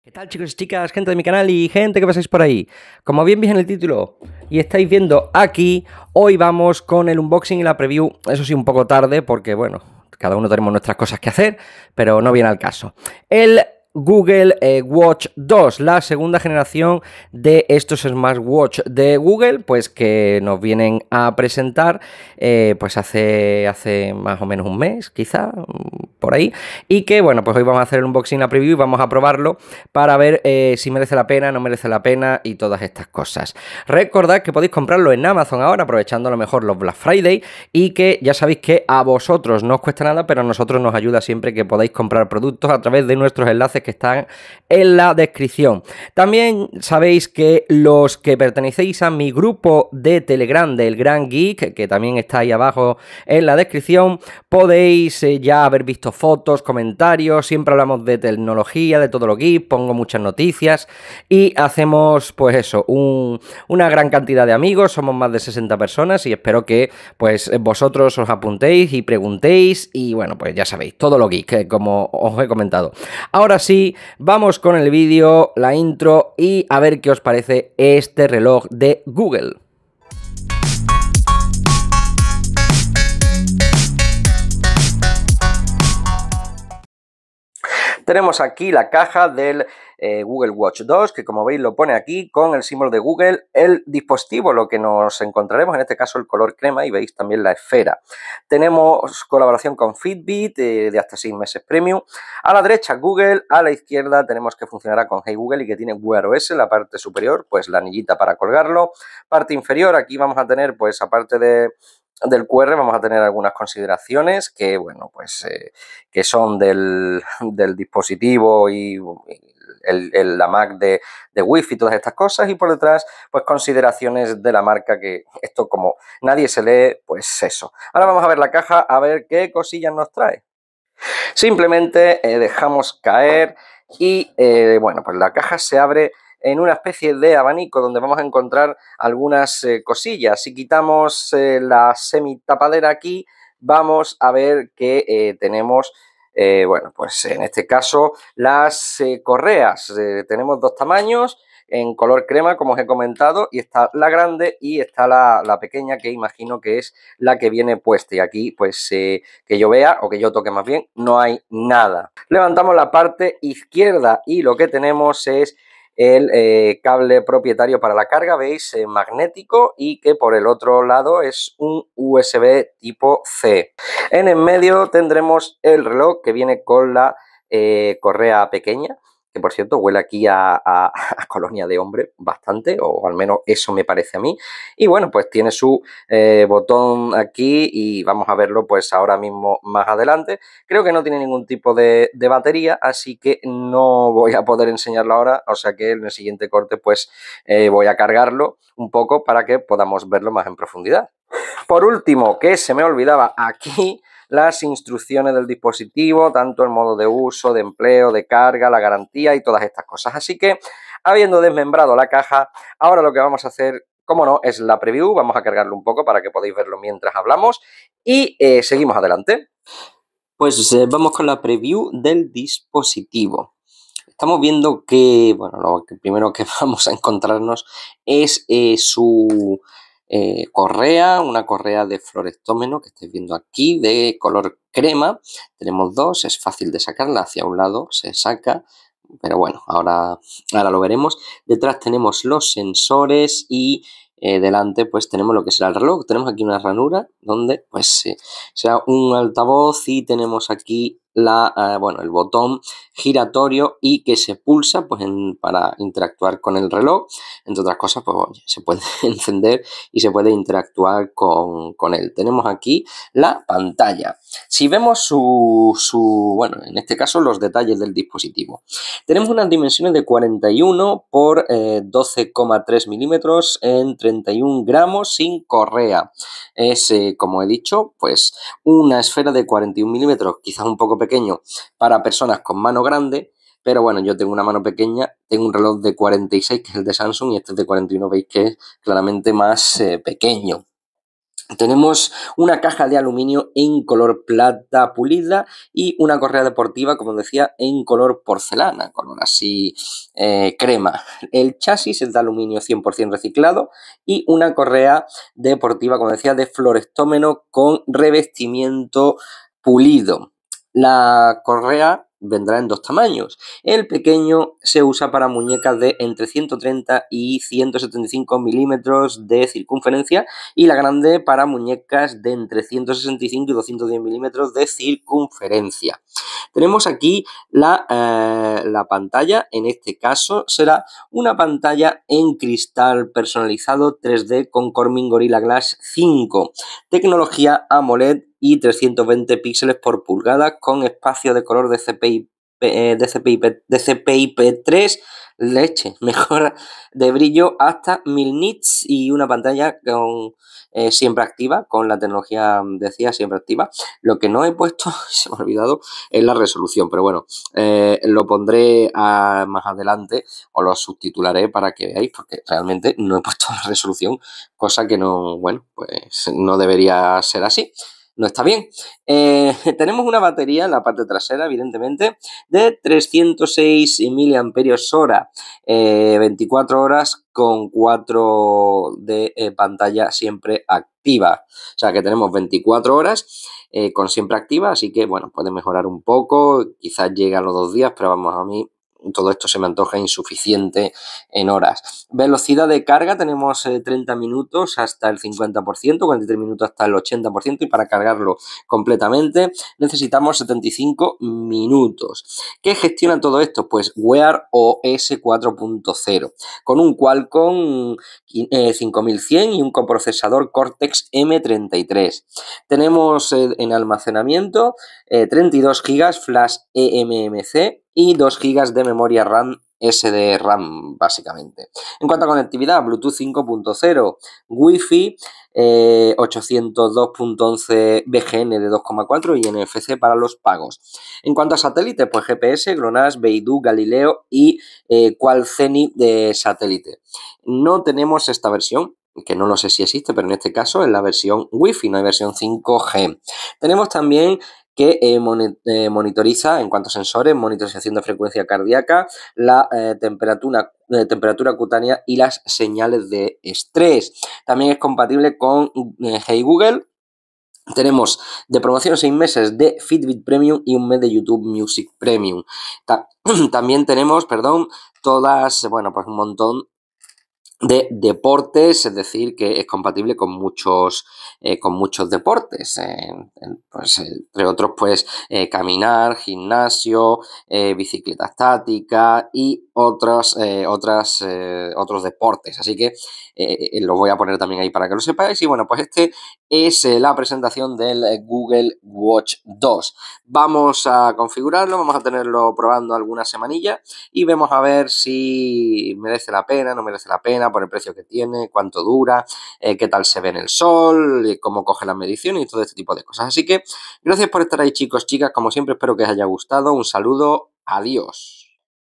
Unboxing y preview del Google Watch 2, tamaño reducido y muchas cosillas [4K@60]
Tenemos el nuevo smartwatch de @GoogleEspana , se trata de la 2nd generación, a ver que nos ofrece!

Ya sabéis que si queréis comprar este y otros artículos, por favor hacedlo a través de nuestro enlace en Amazon